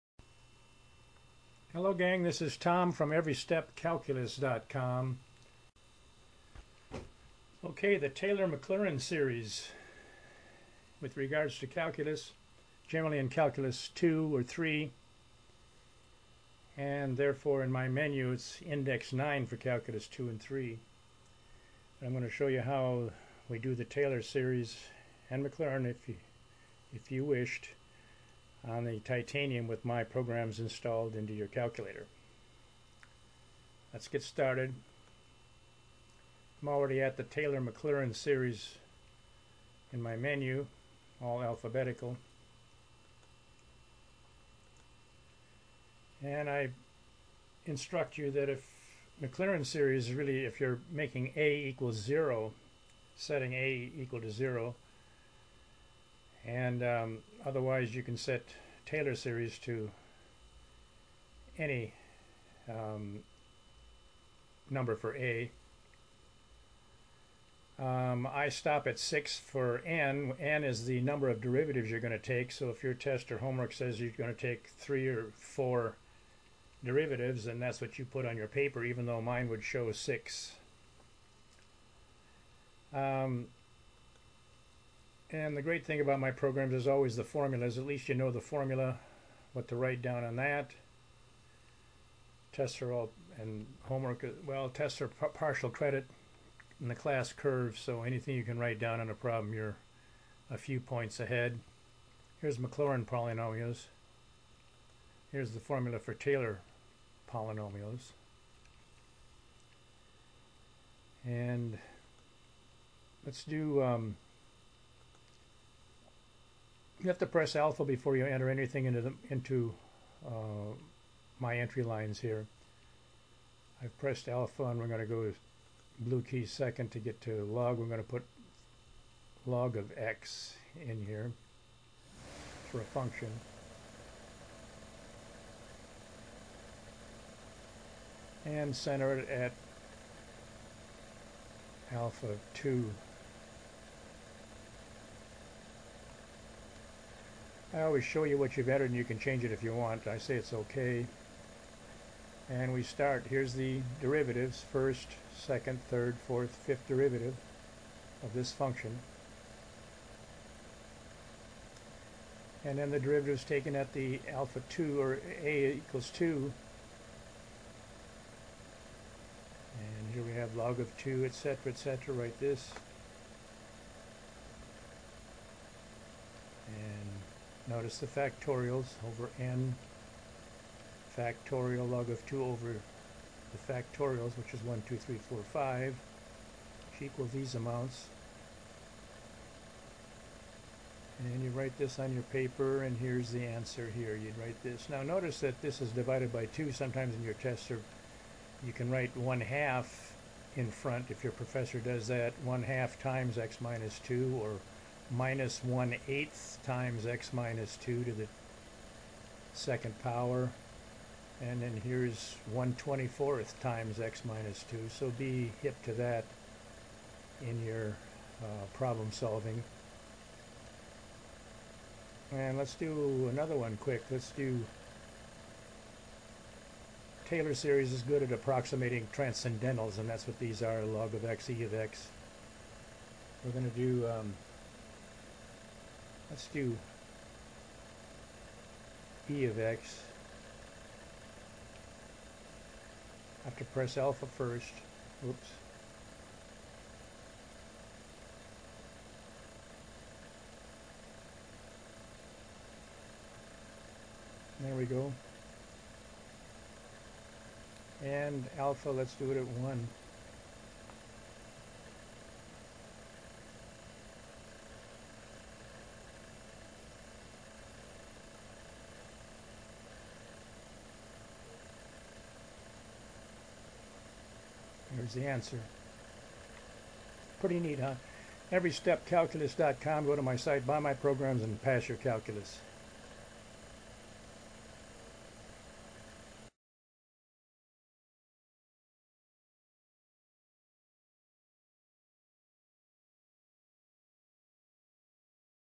taylor series calculator
Watch how the App solves a Limit problem step by step from a real calculus final.